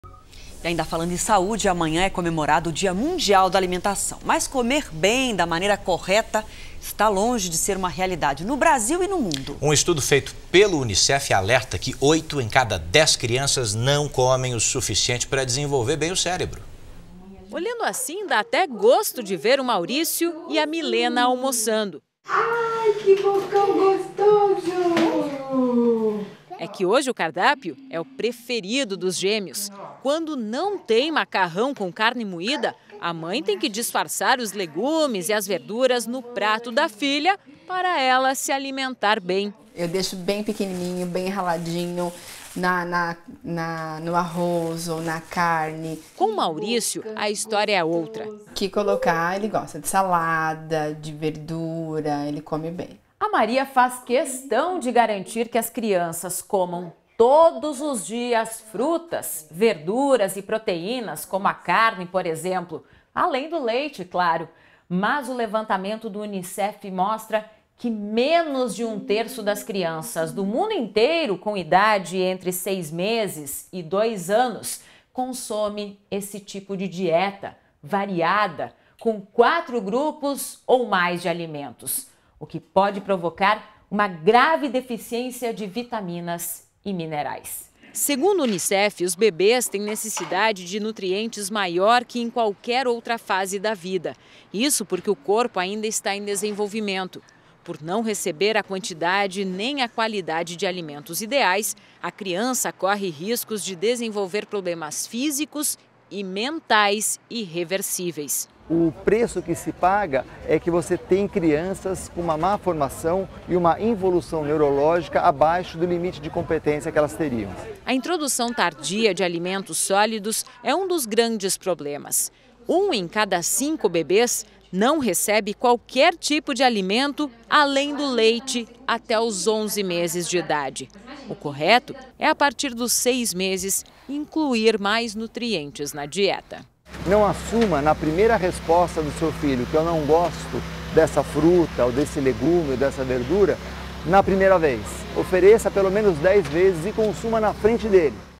Estudo da Unicef alerta que 8 em cada 10 crianças não comem o suficiente
Segundo o estudo da Unicef, oito em cada dez crianças não se alimentam o suficiente para terem um bom desenvolvimento cerebral. A introdução tardia de alimentos sólidos é um dos grandes problemas, um em cada cinco nenéns vivem até os 11 meses apenas com o leite.